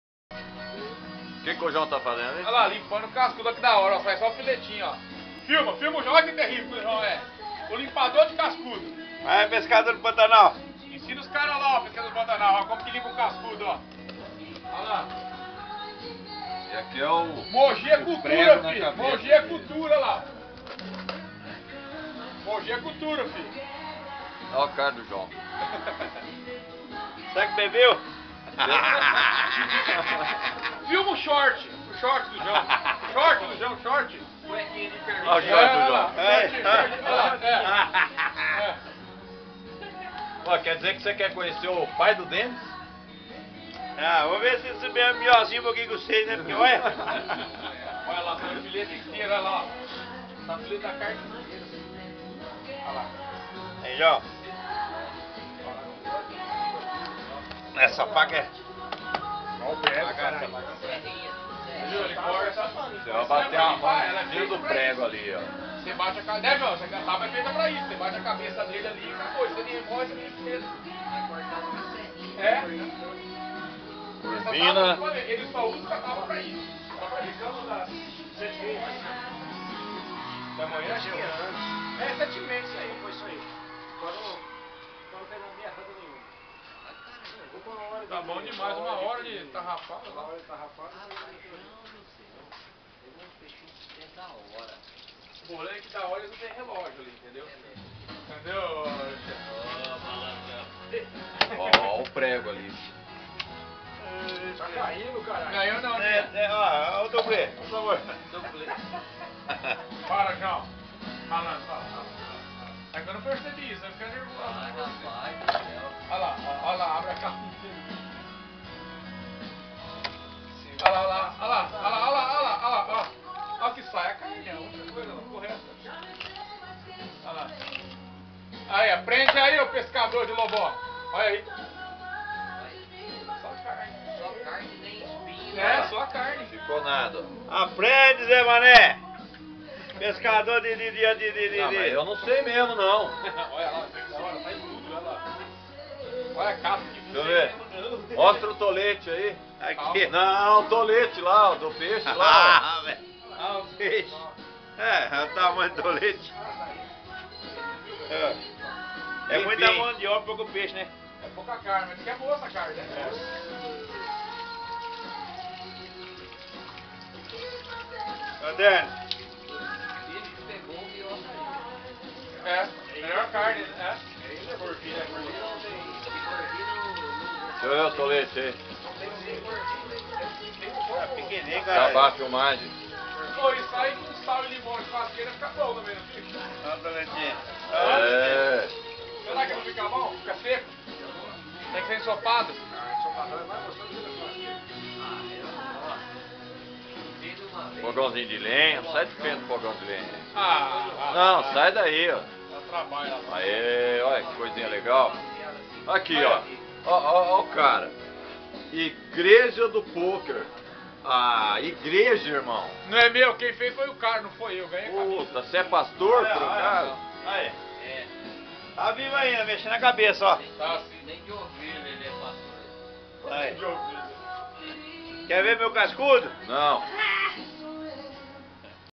limpar cascudo na chacara do joao
joao pescador ensinou assim , para limpar um cascudo pegue uma tabua com um prego grande como mostra no video , fixando a cabeça , com cortes nas costas ao redor do mesmo , a posta do cascudo sai inteira ,